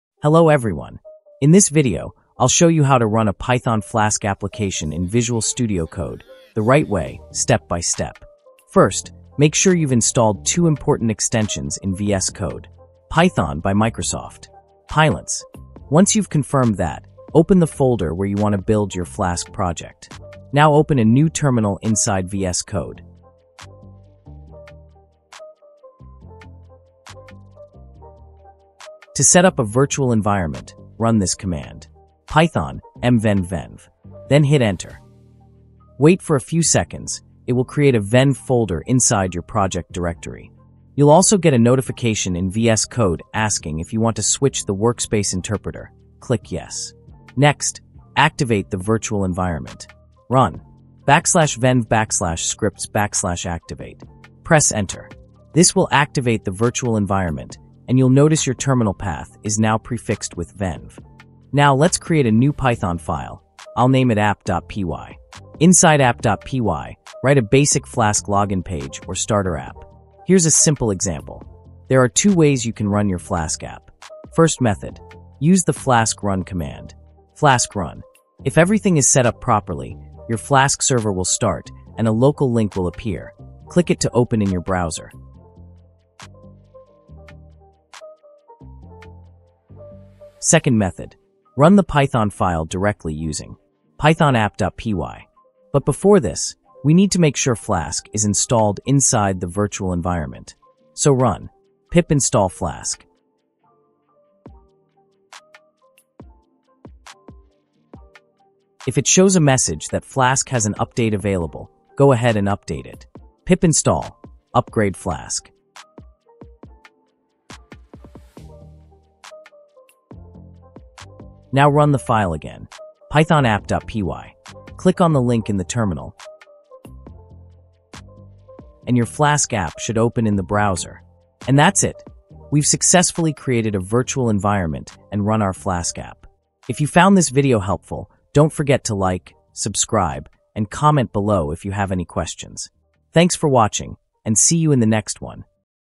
How to Install Flask in Visual Studio Code | Run flask Application in VS Code
Welcome to our latest video where we demonstrate how to install Flask in Visual Studio Code and run a Flask application seamlessly. This tutorial is designed for developers of all skill levels, providing clear instructions and practical tips to enhance your coding experience. We will walk you through the installation process, necessary configurations, and how to execute your Flask application within the VS Code environment. Join us as we explore the powerful features of Flask and Visual Studio Code together!